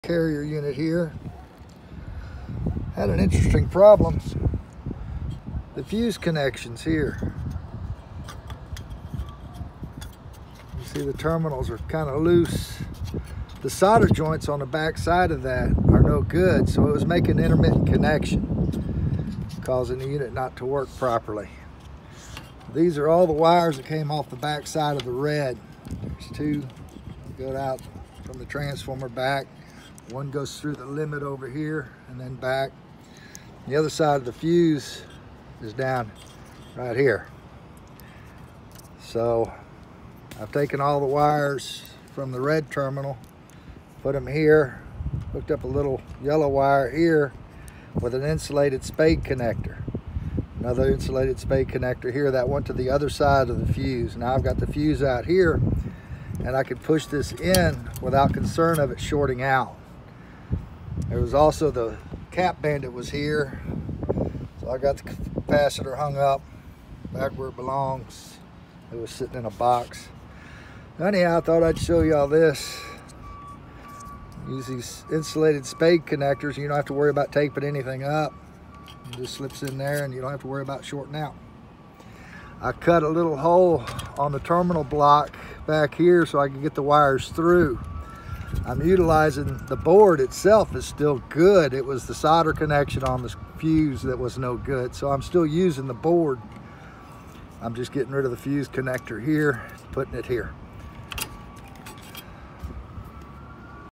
Control problem
This unit was cycling when it should have been running. While troubleshooting the unit at the thermostat connections it started chattering when I had my test lead on the fuse. I shut down the unit and found the solder connection for the fuse connector had come loose. This is how to repair this kind of issue. Stay safe.